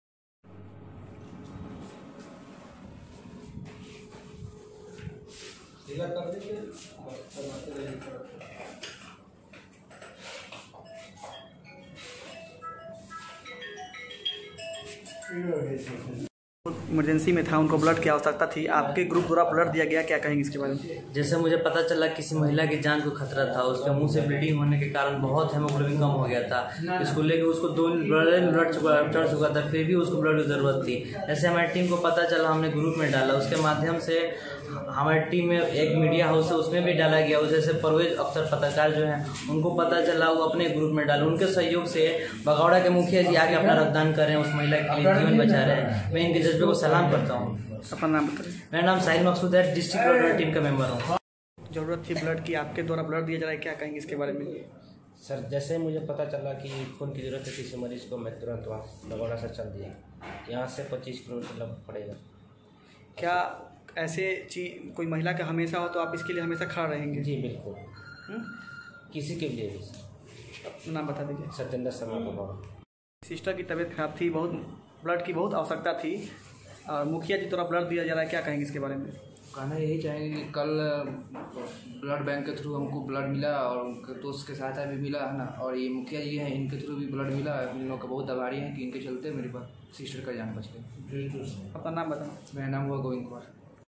सत्येंद्र शर्मा ने रक्त देकर बचाई महिला की जान. ब्लड बैंक में ब्लड की कमी मरीज परेशान. सिवान
सरकार न्यूज़ लाइव

अमित कुमार मोनू की रिपोर्ट